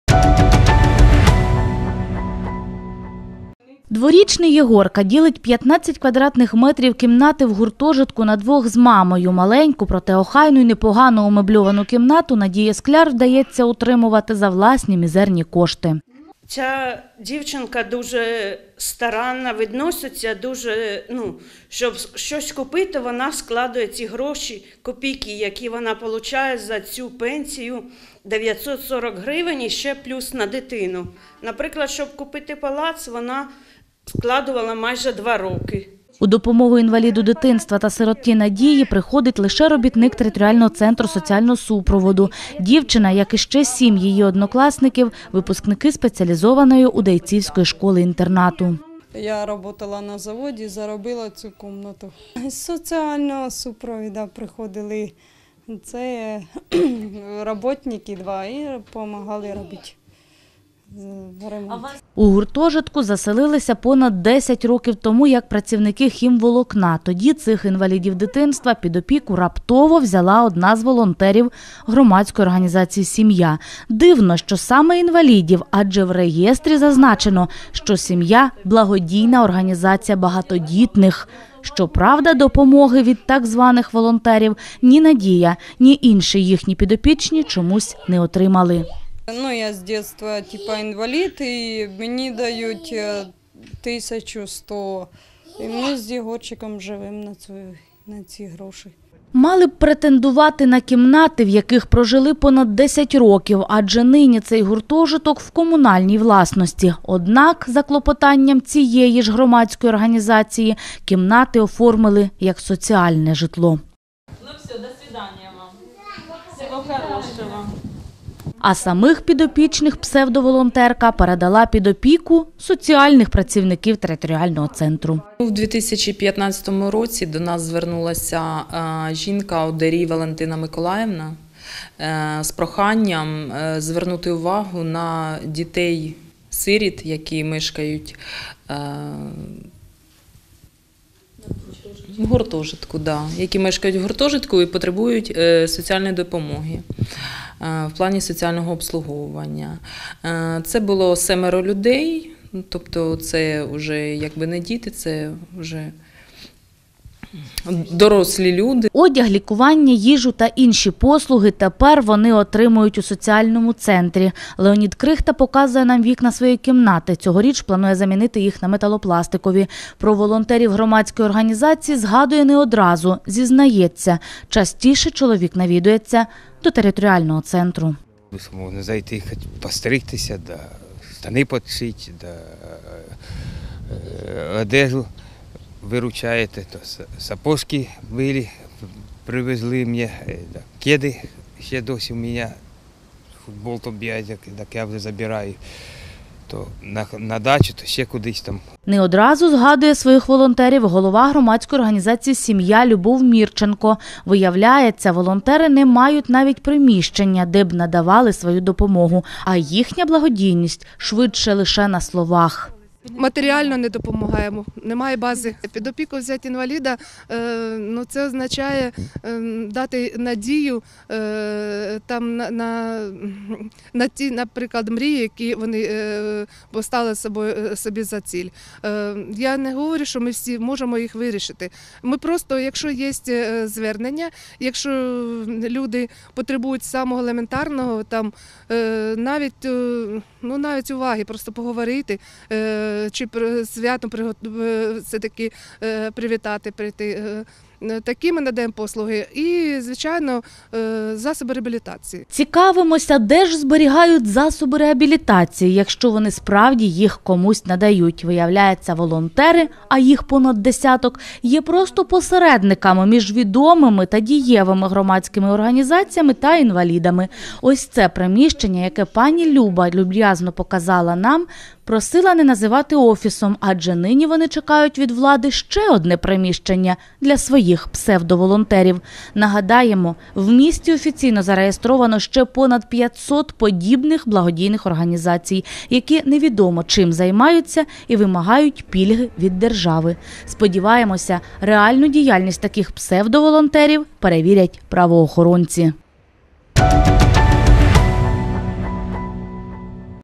Чернигов: Обогащающая опека: откуда берутся псевдоволонтеры и за счет чего работают
Двухлетний Егорка делит с мамой пятнадцать квадратных метров комнаты в общежитии. Маленькую, но опрятную и не плохо меблированную комнату Надежде Скляр удается содержать за собственные мизерные денежные средства